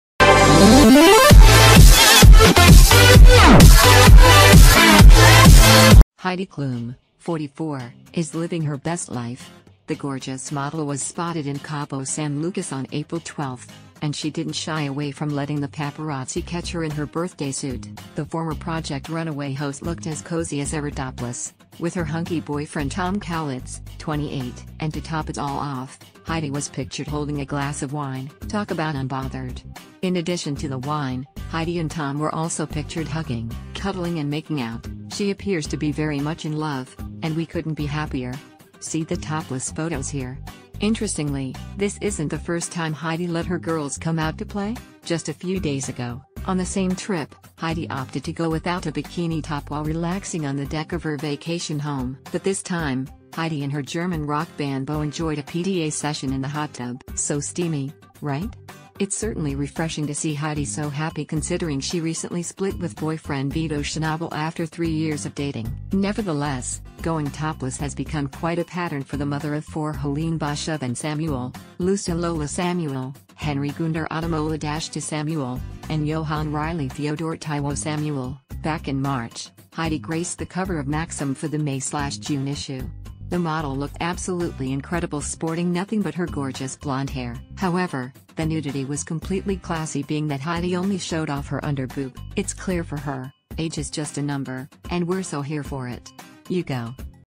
Heidi Klum Vacations Topless With Hot New Boyfriend In Cabo San Lucas: See Pics
Heidi Klum, 44, is living her best life! The gorgeous model was spotted in Cabo San Lucas on April 12, and she didn’t shy away from letting the paparazzi catch her in her birthday suit. The former Pro...